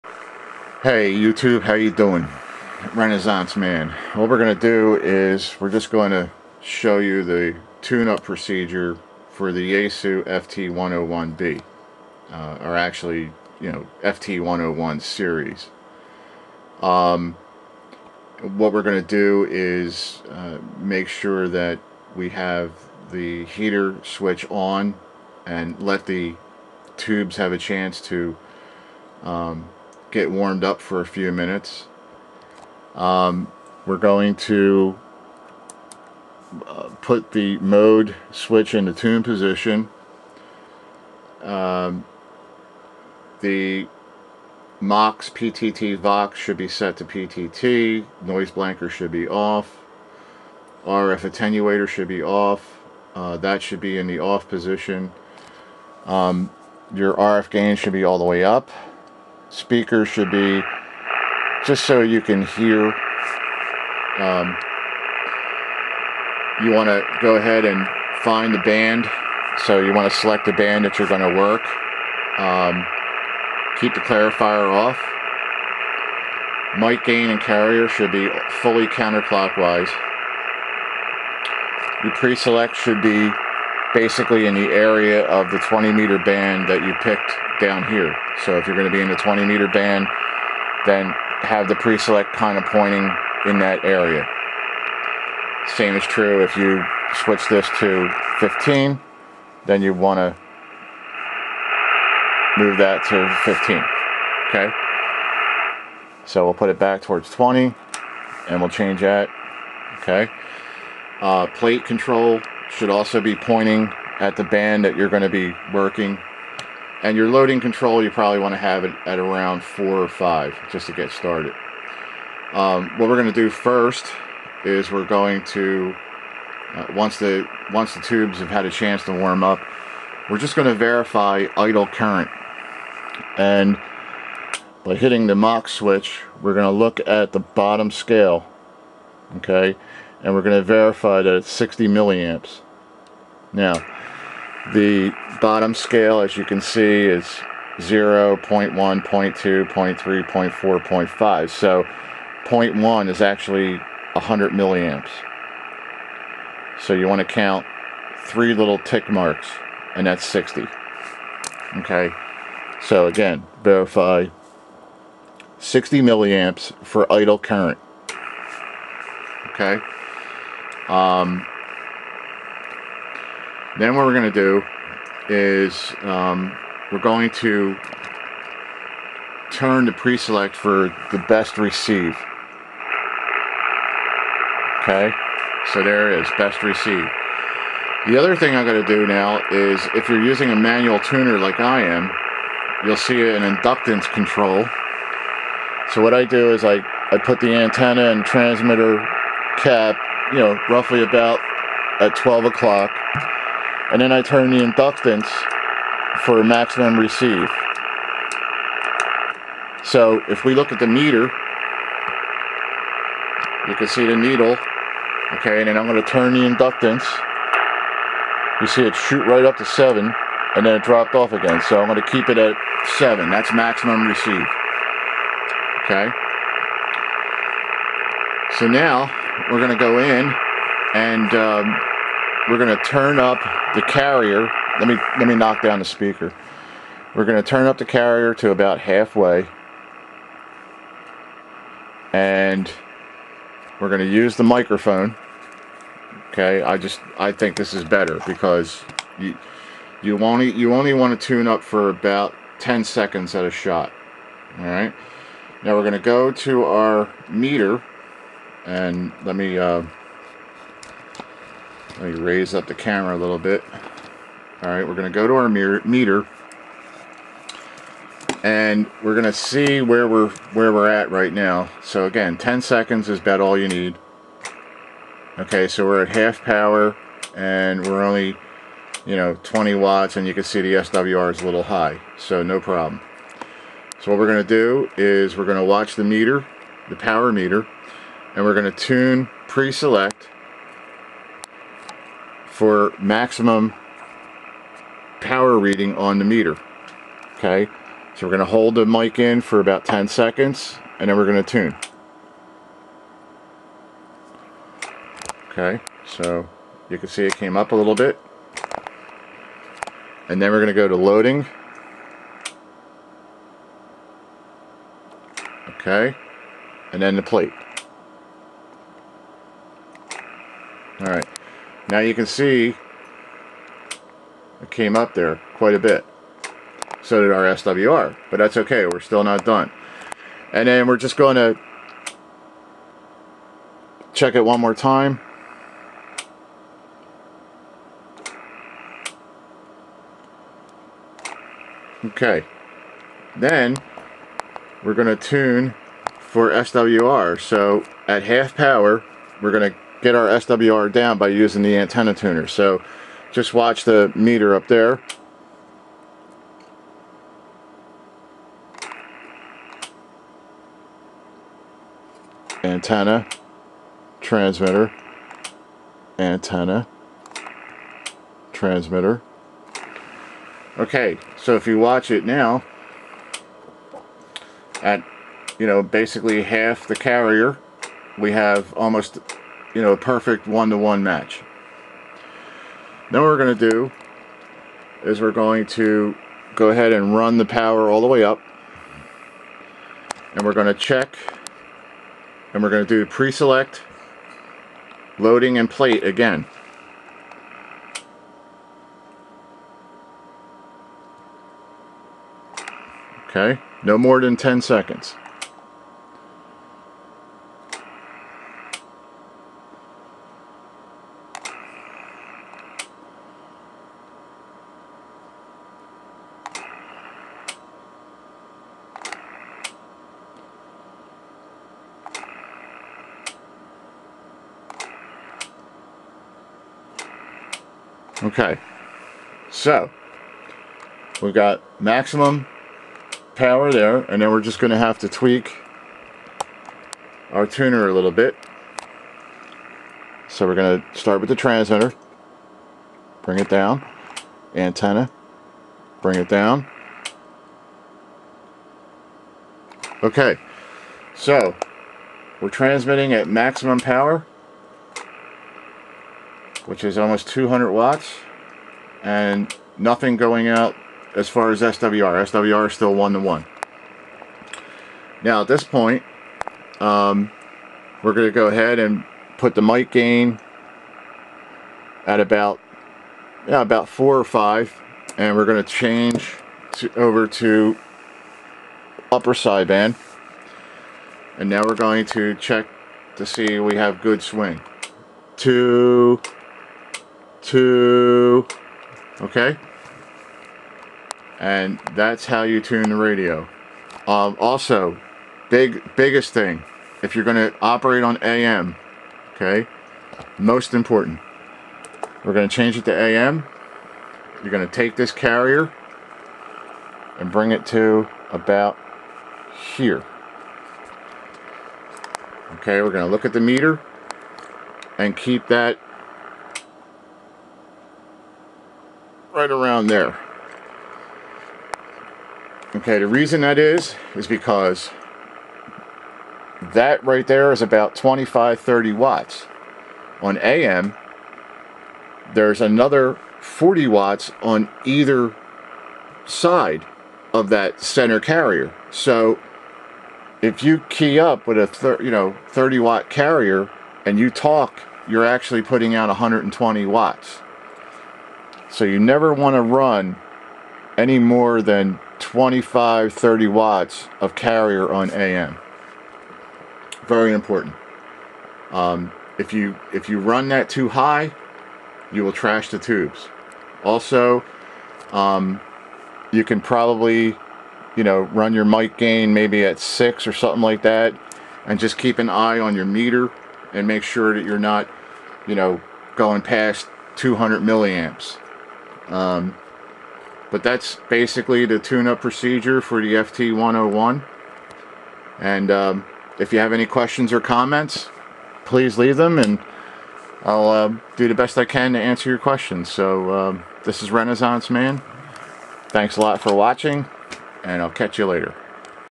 Yaesu FT-101B; Tune Up Procedure
HamRadio In this video, we will cover how to tune up an ft-101. There are a few ways to do this, however, I'm showing you how I tune up my radio.